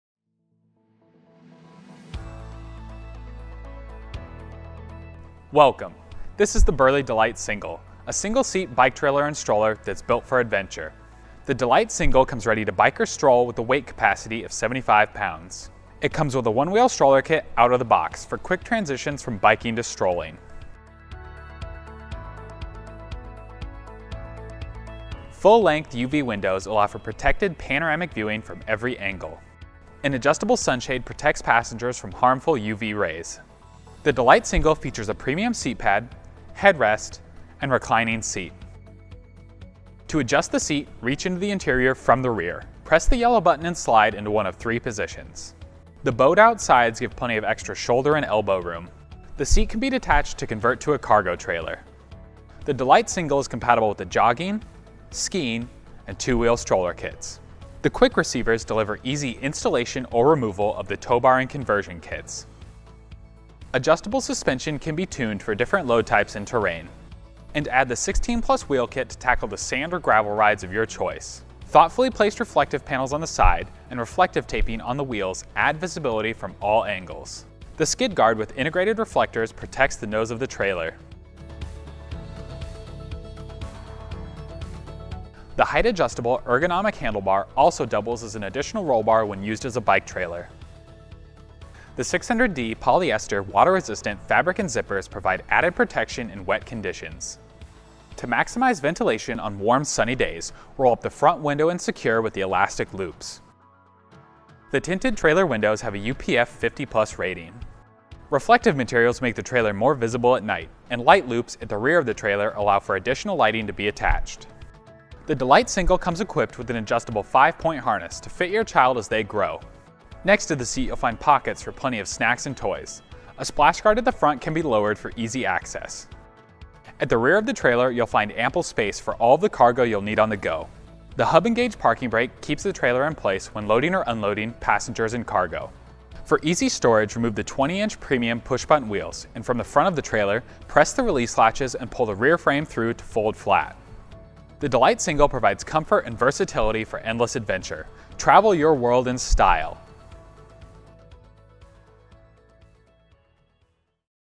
Burley DLite Single Anhänger
Der Einplätzer Burley Fahrradanhänger D’Lite ist für abenteuerliche Familien der perfekte Begleiter. Der D’Lite Single bietet einen cleveren Innenbereich mit viel Komfort. Weitere Informationen bei 4mybaby.ch!